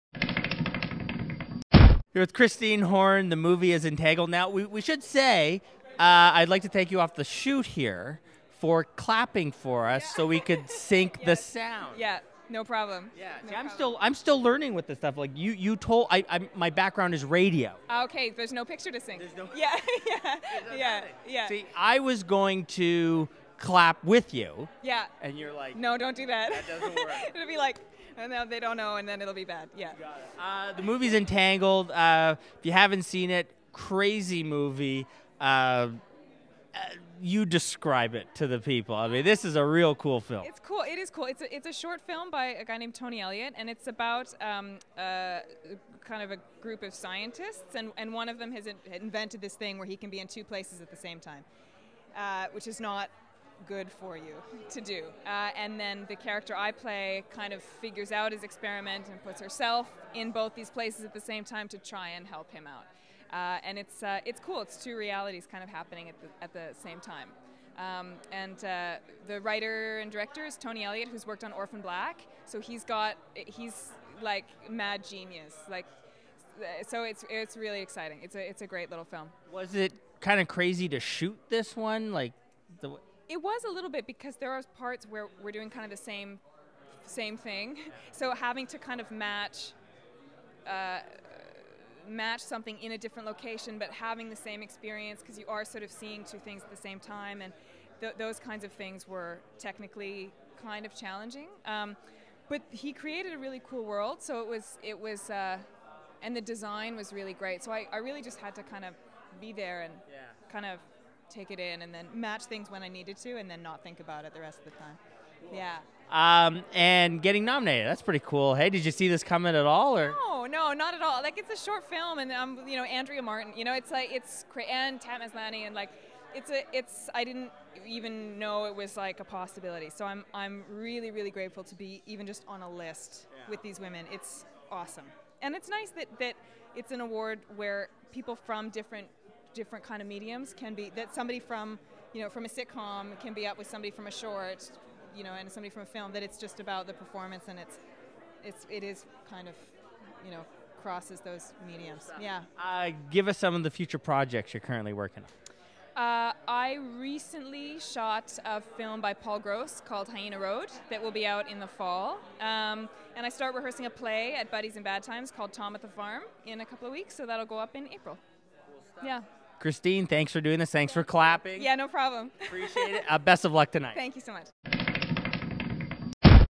Christine Horne Interview at 2015 ACTRA Awards
Christine Horne was nominated for her role in Entangled at the 2015 ACTRA's.  Mr ShoBiz was lucky enough to interview her!